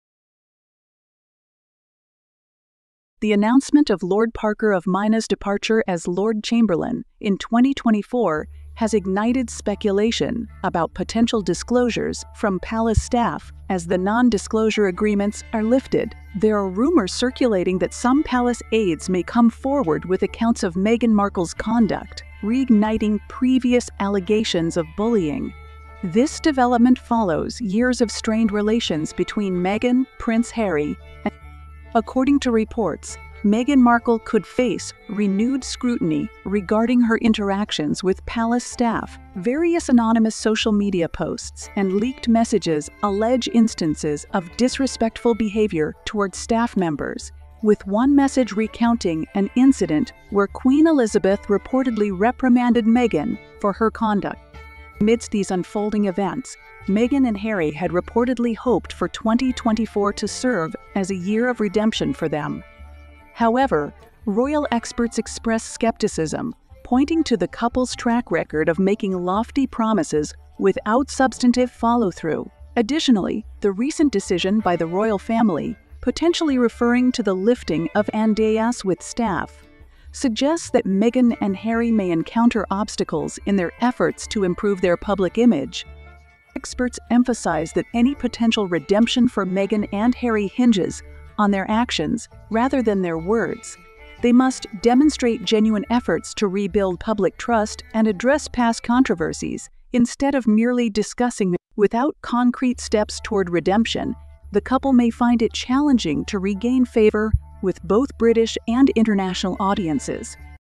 Lord Parker lifts NDA for W personnel in 2024; Meg reacts crazily to the shocking new year start.
The announcement of Lord Parker of Mina's departure as Lord Chamberlain in 2024 has ignited speculation about potential disclosures from palace staff as the Non-Disclosure Agreements (NDAs) are lifted. There are rumors circulating that some palace aides may come forward with accounts of Meghan Markle's conduct, reigniting previous allegations of bullying. This development follows years of strained relations between Meghan, Prince Harry, and the royal family.

According to reports, Meghan Markle could face renewed scrutiny regarding her interactions with palace staff. Various anonymous social media posts and leaked messages allege instances of disrespectful behavior towards staff members, with one message recounting an incident where Queen Elizabeth reportedly reprimanded Meghan for her conduct.

Amidst these unfolding events, Meghan and Harry had reportedly hoped for 2024 to serve as a year of redemption for them. However, royal experts express skepticism, pointing to the couple's track record of making lofty promises without substantive follow-through. Additionally, the recent decision by the Royal Family, potentially referring to the lifting of NDAs with staff, suggests that Meghan and Harry may encounter obstacles in their efforts to improve their public image.

Experts emphasize that any potential redemption for Meghan and Harry hinges on their actions rather than their words. They must demonstrate genuine efforts to rebuild public trust and address past controversies instead of merely discussing them. Without concrete steps toward redemption, the couple may find it challenging to regain favor with both British and international audiences.